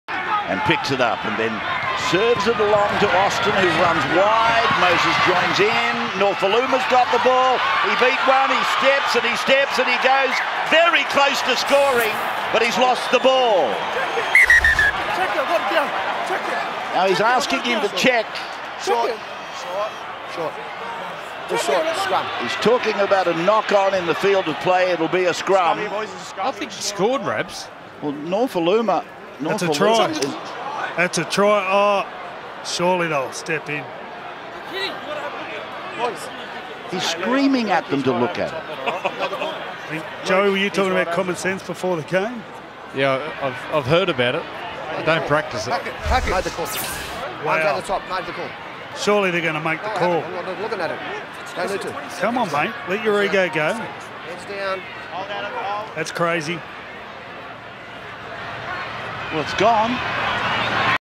NRL 2014: Shayne Hayne robs David Nofoaluma a try
Shayne Hayne failed to use the Video Referee when David Nofoaluma scored a try. This decision shifted the momentum for the whole game.